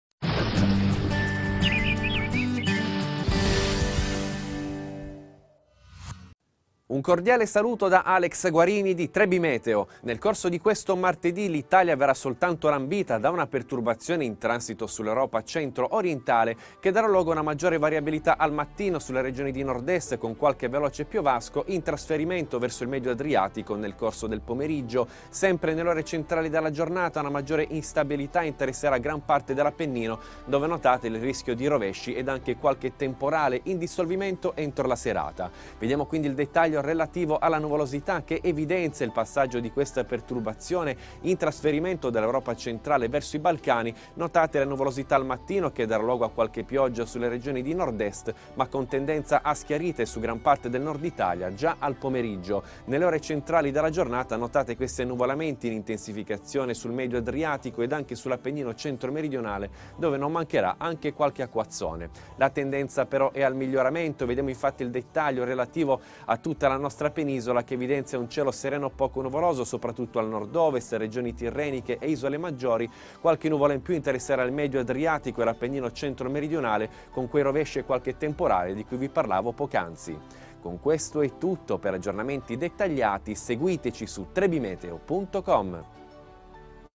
Previsioni meteo Video per martedi, 11 aprile
Previsioni del tempo a cura di Alex Guarini ☼ Vi ringraziamo per i mi piace e per le condivisioni ☼